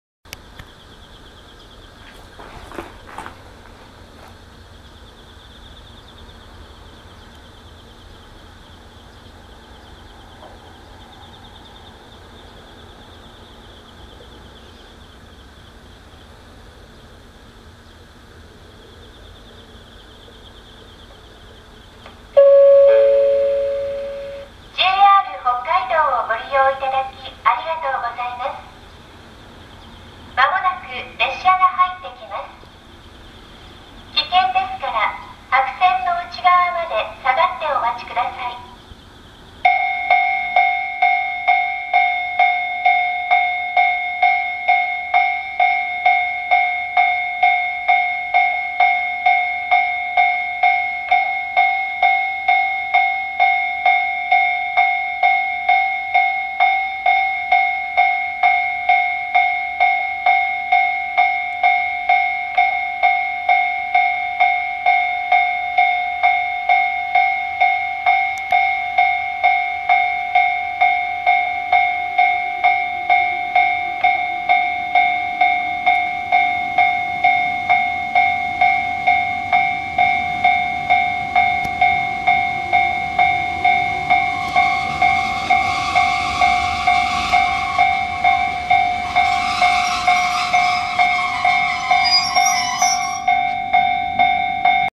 JR北海道721系 普通 岩見沢行き 到着シーン
721系(F-3015編成)
普通 岩見沢行き(2240M)
2015年09月20日 北海道旅客鉄道函館本線 妹背牛駅
到着シーン